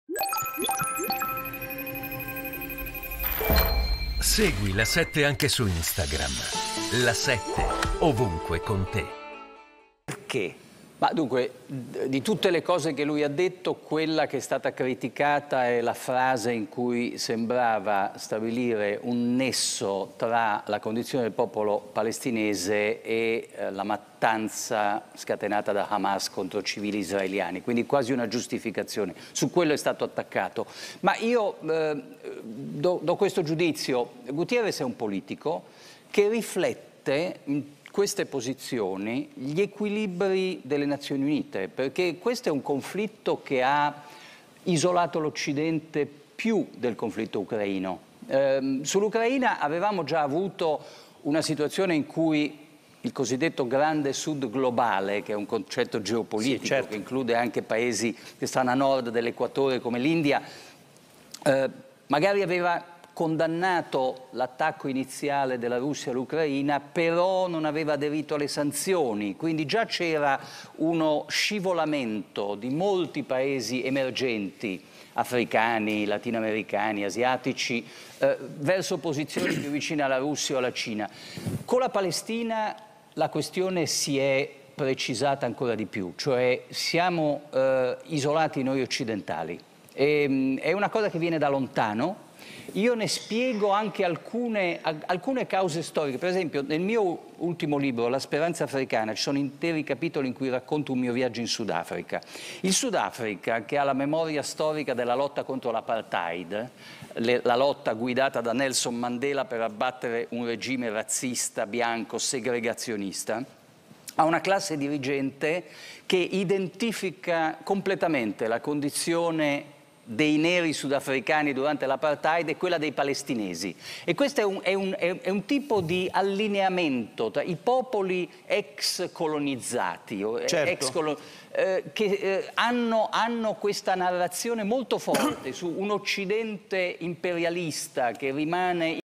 Rampini: «Guterres riflette gli equilibri delle Nazioni Unite»
«Hanno criticato Guterres per la frase in cui sembrava stabilire un nesso tra la condizione del popolo palestinese e la mattanza scatenata da Hamas contro i civili israeliani. Lui è un politico che riflette gli equilibri delle Nazioni Unite perché questo è un conflitto che ha isolato l’Occidente più di quello ucraino». Il commento dell’editorialista del Corriere della Sera, Federico Rampini.